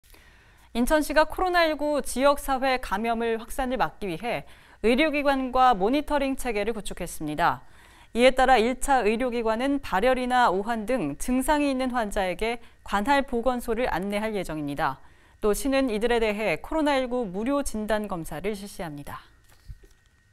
인천시, 의료기관과 모니터링 체계 구축
[인천뉴스 이정윤 기자]

[기사내용]

인천시가

코로나19 지역사회 감염 확산을 막기 위해

의료기관과 모니터링 체계를 구축했습니다.

이에 따라 1차 의료기관은

발열이나 오한 등 증상이 있는 환자에게

관할 보건소를 안내할 예정입니다.

또 시는 이들에 대해

코로나19 무료 진단 검사를 실시합니다.